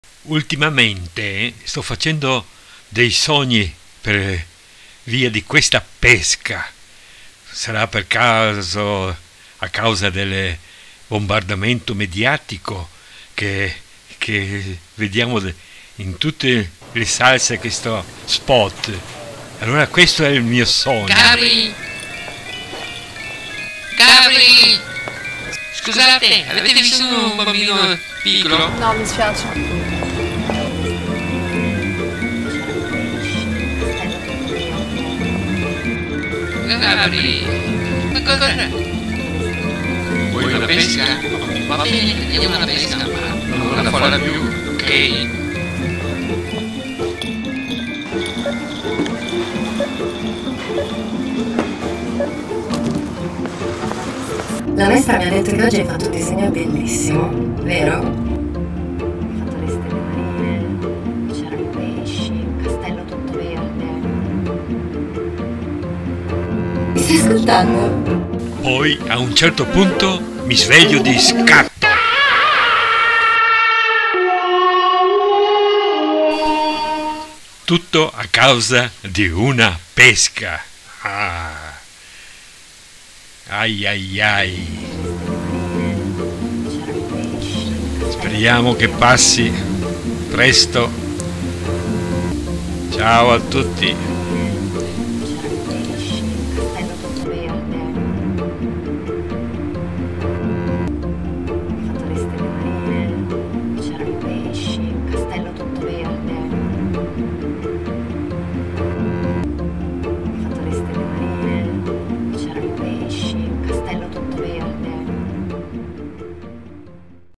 L'INCUBO della PESCA di ESSELUNGA... sogno o son desto?
A forza di sentir parlare di questo spot, di vederlo e rivederlo in continuazione provoca degli incubi durante la notte.